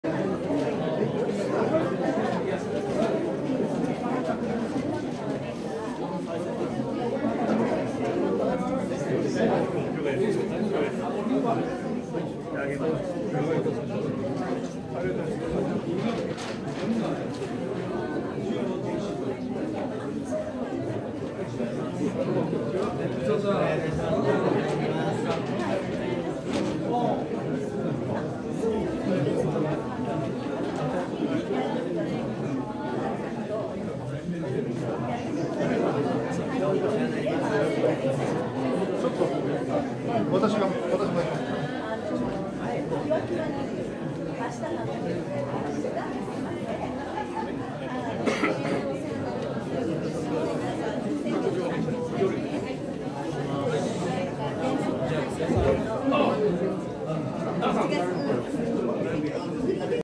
節分会
今年もかみしも着せていただきます。男性の力じゃないとピシッとしません‼️今年も撒くよ撒くよー福は内🎵